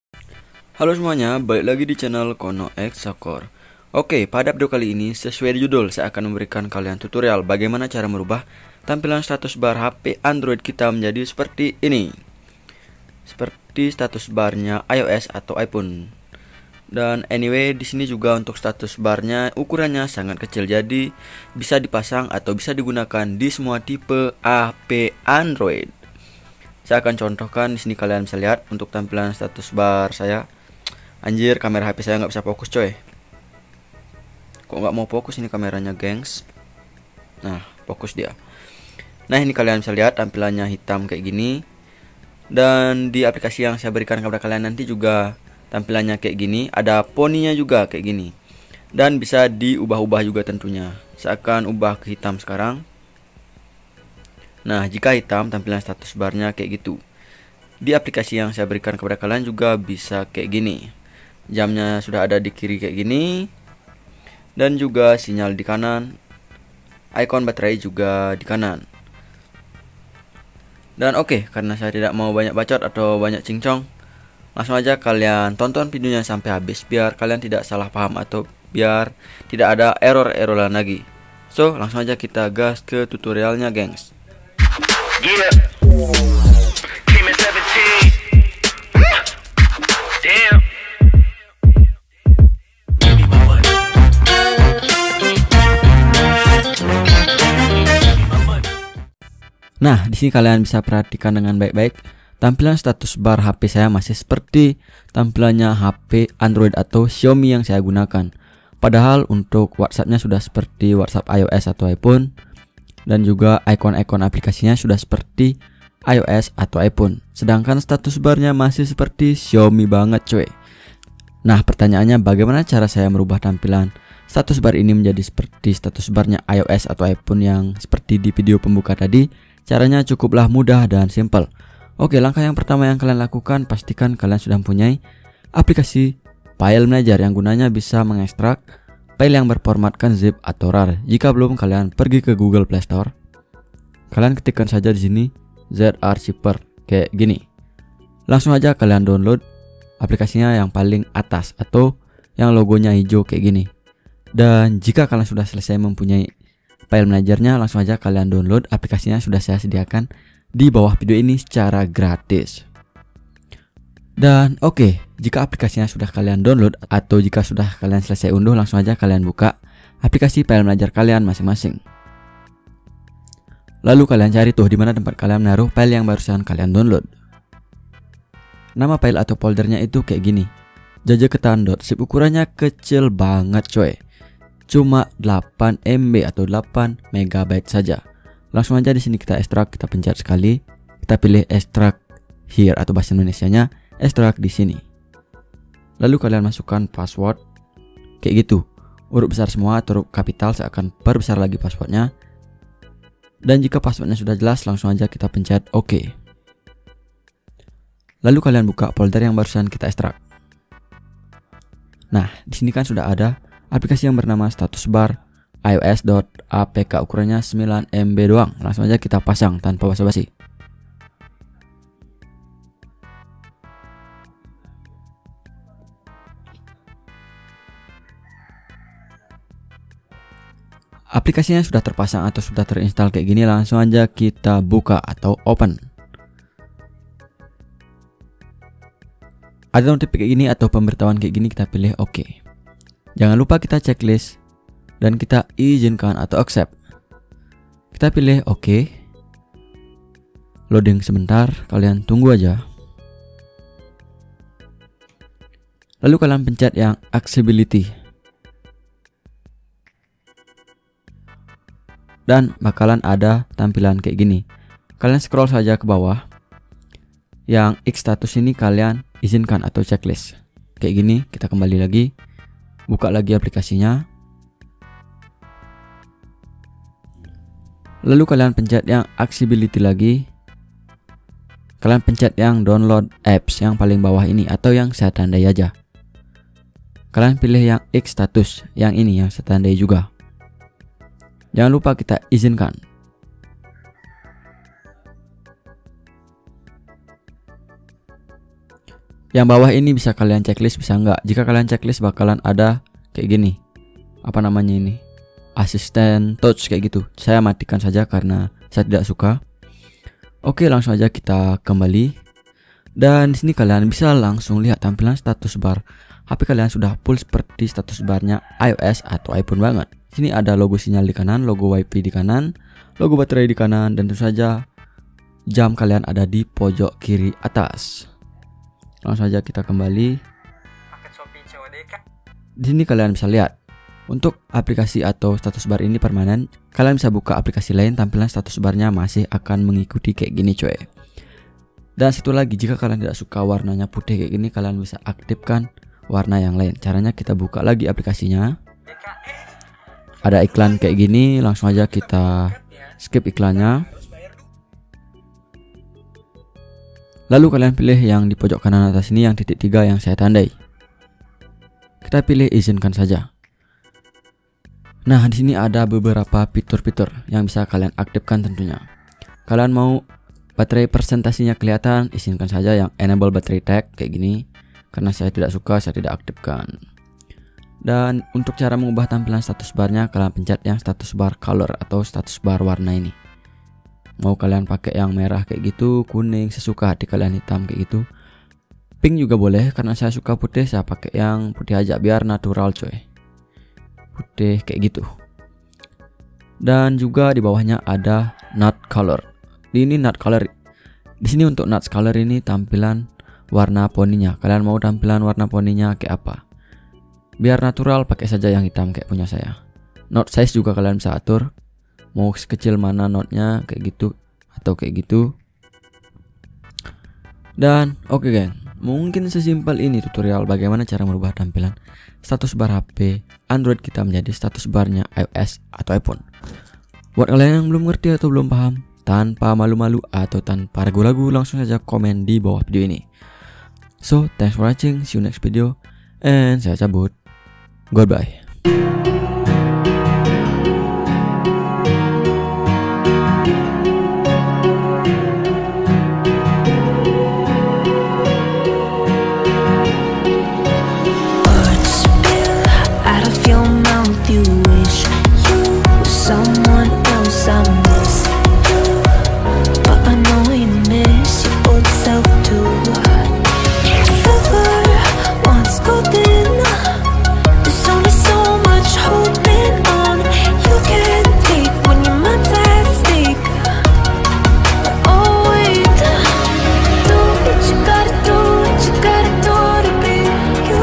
cara ubah status bar android menjadi iPhone work semua android terbaru 2022
Hallo kawan kawan ku semuanya pada video kali ini aku akan memberikan tutorial bagaimana cara merubah status bar android kalian menjadi seperti status bar iPhone terbaru, nah untuk full review nya kalian cek video di atas, untuk link nya ada di bawah ini yak 👇👇👇👇